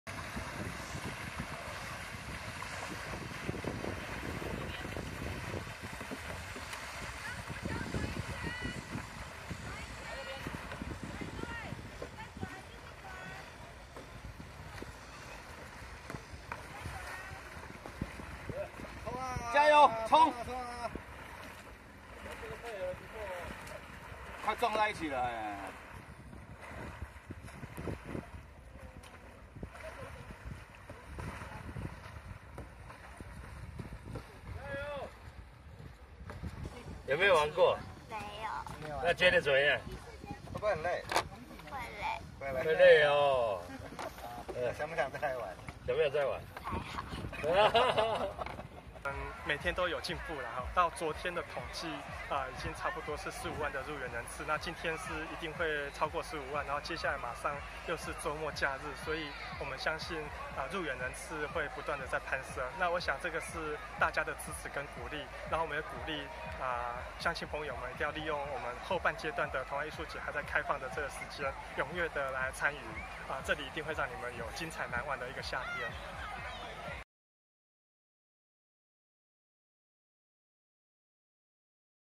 搭帆船、划獨木船、龍舟 宜蘭童玩節新玩法
相關新聞連結:
本月7日開幕的宜蘭國際童玩藝術節逐漸升溫，入園累計人數已超過15萬人次，遊客除了觀賞外國團隊表演、戲水、參觀主題展館，冬山河水域還有新玩法，遊客可搭帆船、划獨木船、划龍舟，體驗水上駕舟的樂趣。